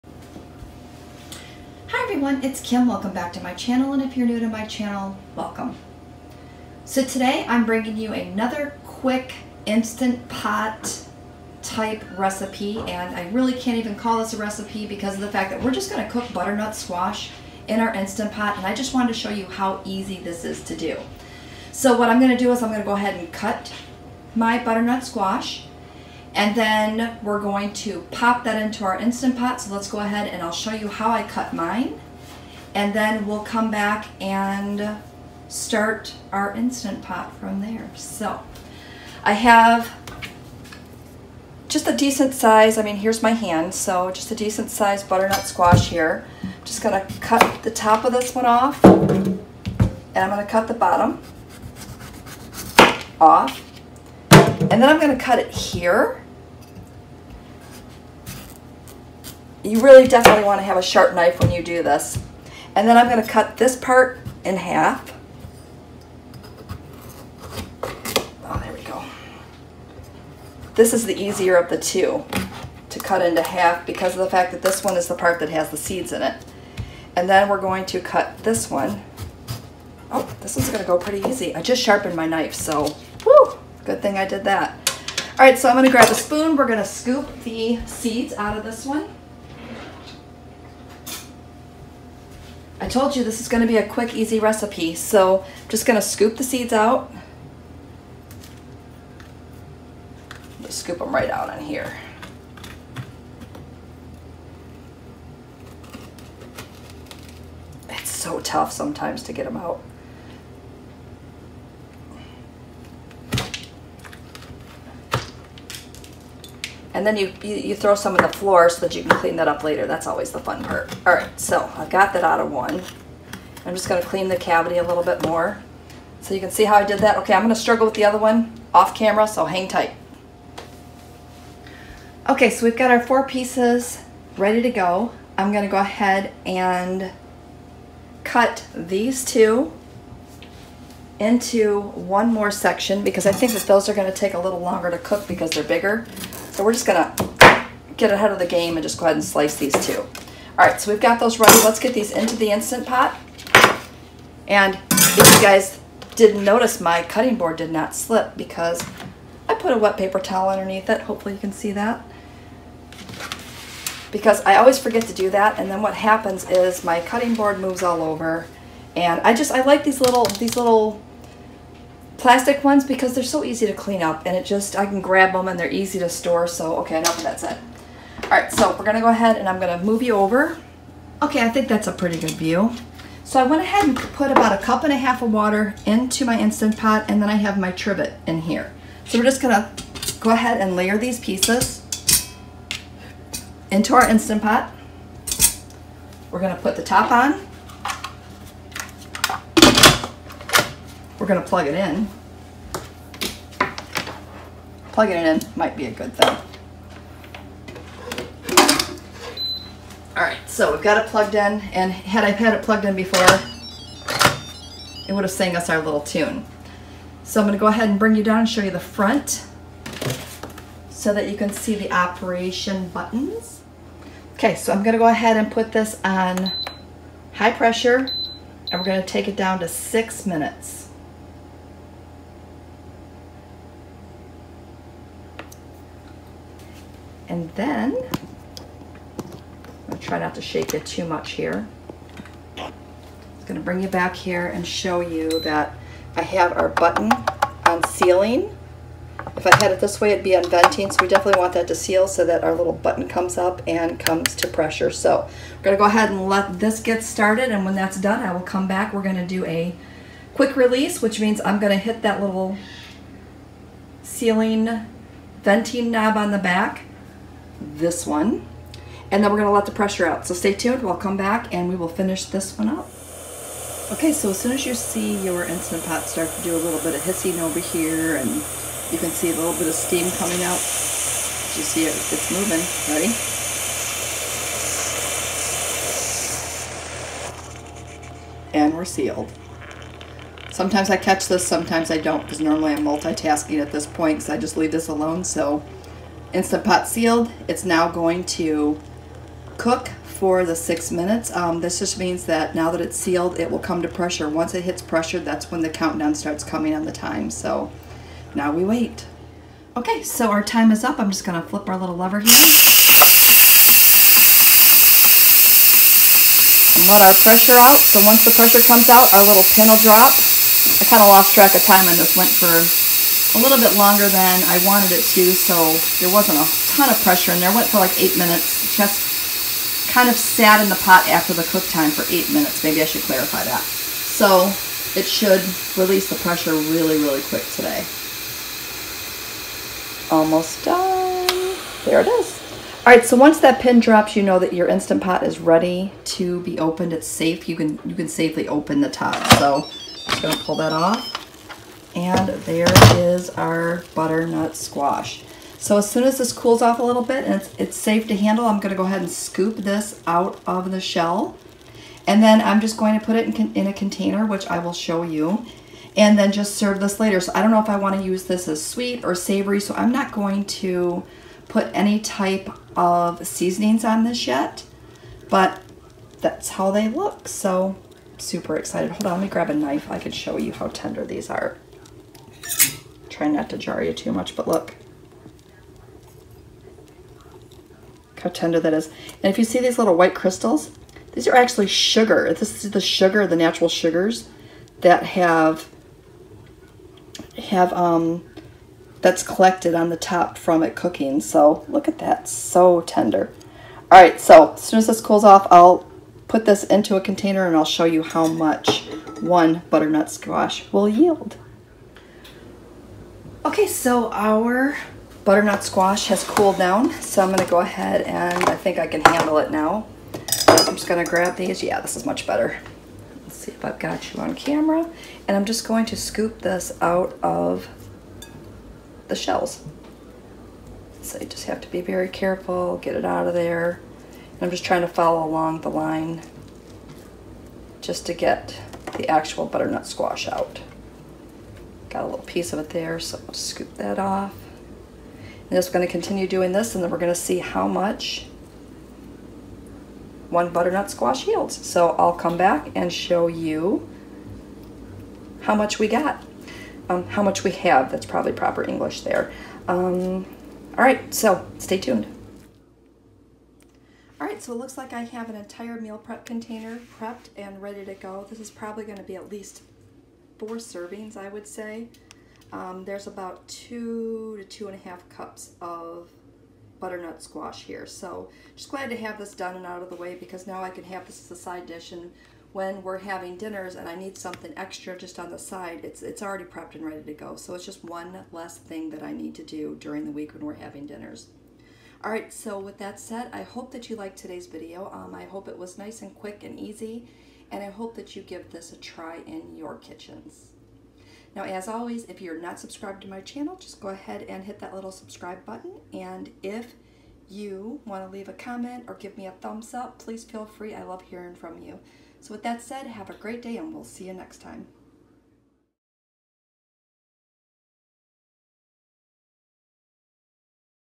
Instant Pot Butternut Squash - MyWW
So quick and so easy !!
Definitely pull out your Instant Pot and give this one a try !!

Tune into my channel for weekly, WW friendly; cook with me, meal prep, or food related videos.

Let’s have some fun while staying on track with WW.